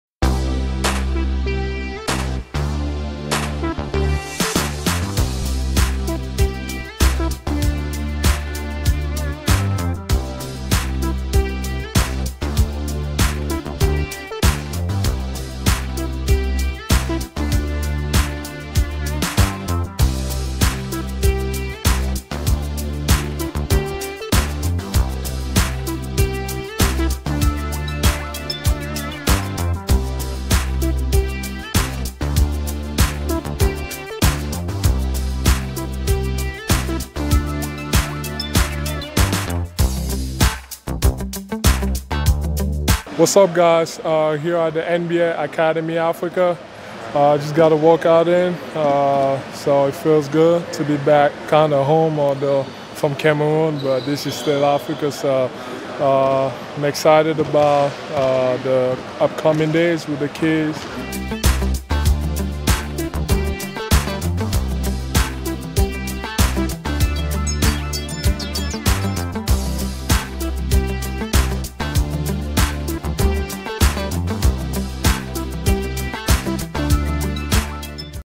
Le basketball africain n'a plus de limite ! 🏀
À Saly, au Sénégal, la NBA vient entraîner de jeunes athlètes venus de toute l'Afrique ! 🏀